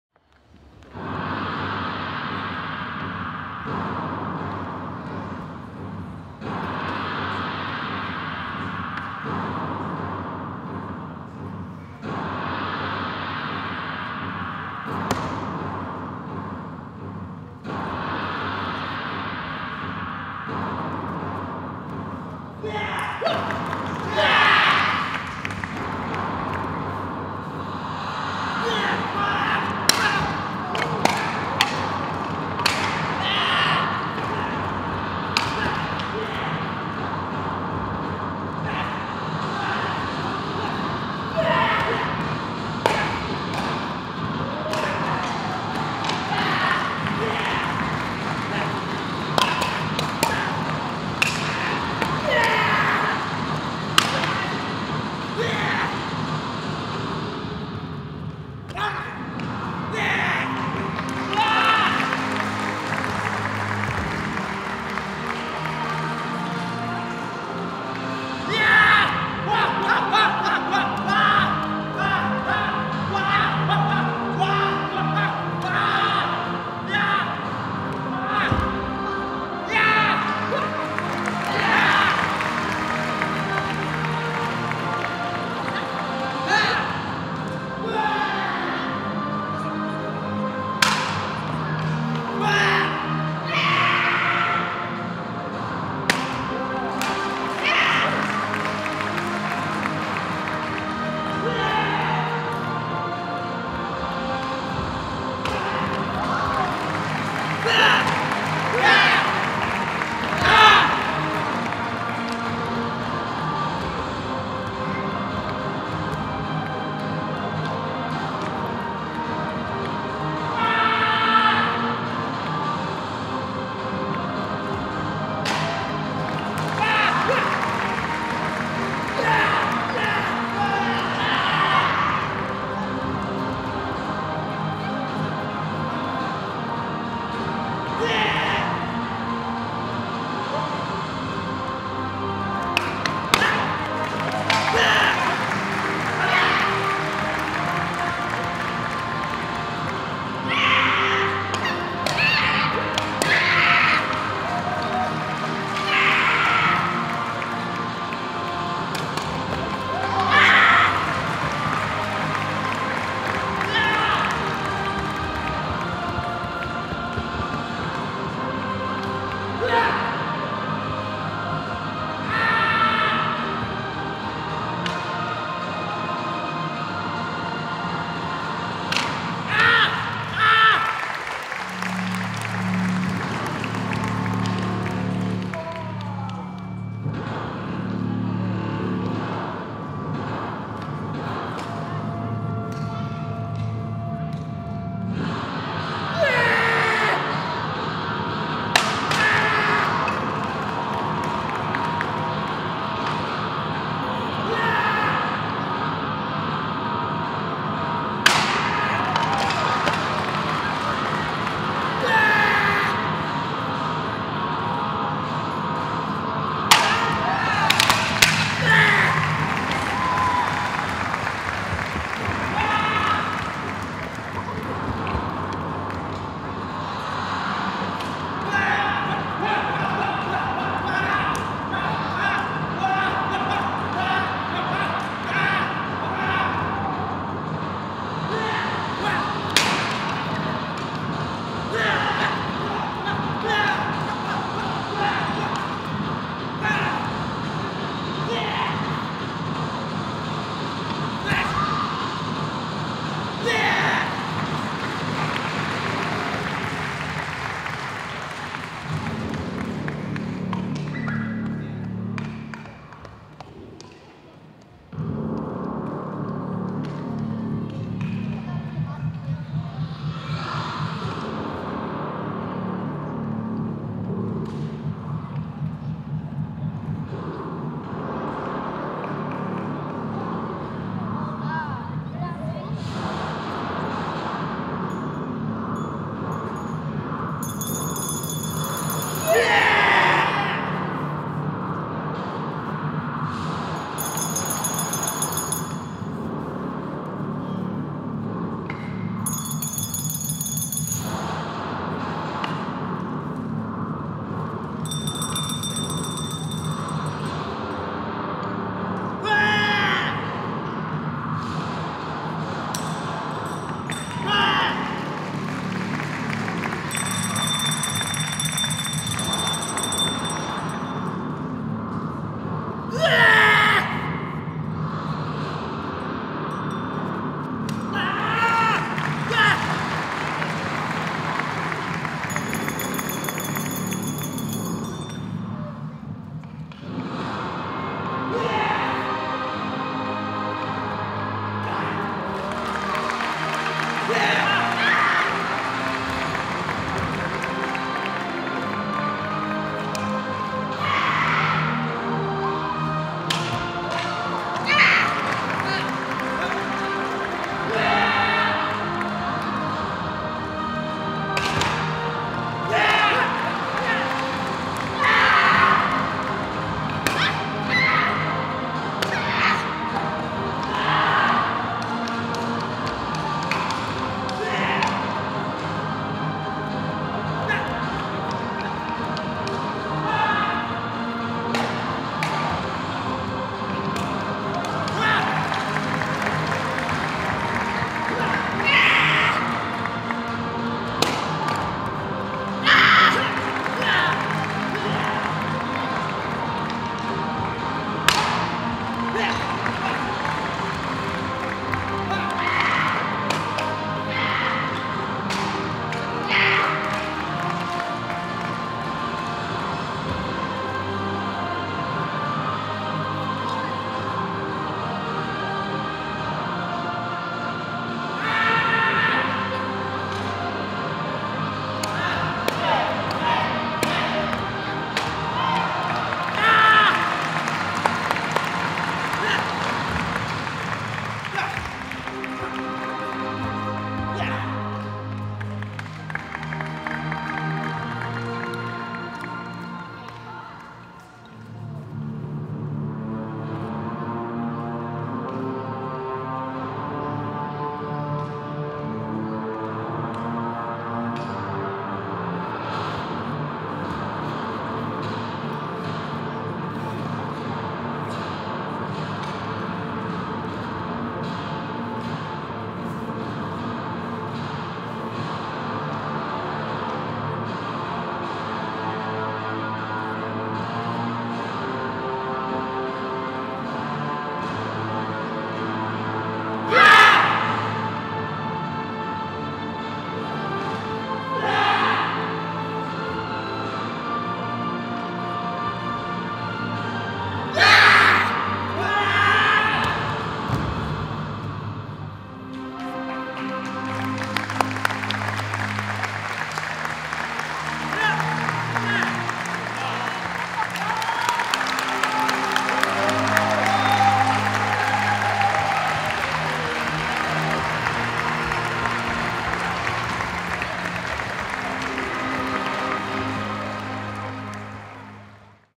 MOOHWA Taekwondo Demo Team - HANMADANG 2022
Showeinlage vom MOOHWA Taekwondo Demo Team vom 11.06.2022 im Rahmen des HANMADANG 2022 in der Sporthalle in Tulln.